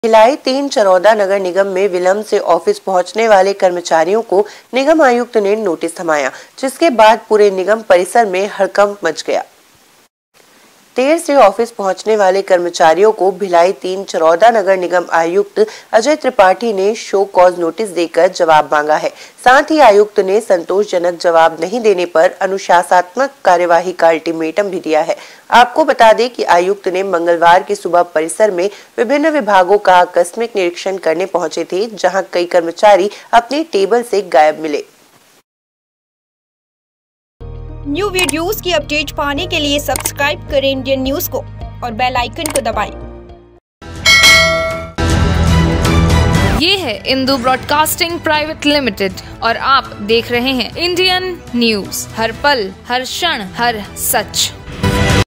भिलाई : नगर निगम आयुक्त की बड़ी कार्यवाही, देर पहुंचने वाले कर्मचारियों को थमाया नोटिस
Indian News :  भिलाई तीन चरोदा नगर निगम में विलंब से ऑफिस पहुंचने वाले कर्मचारियों को निगम आयुक्त ने नोटिस थमाया, जिसके बाद पूरे निगम परिसर में हडकंप मच गया।

Our Links